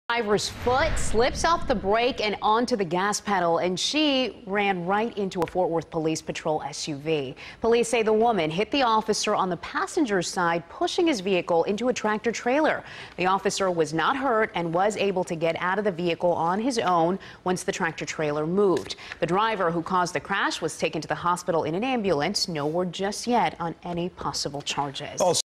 Police: Woman Wearing 'Greasy' Shoes T-Bones Police SUV After Foot Slips Off Brake
A woman who was wearing "greasy, slippery" shoes crashed into a Fort Worth police vehicle after her foot slipped off the brake, authorities say.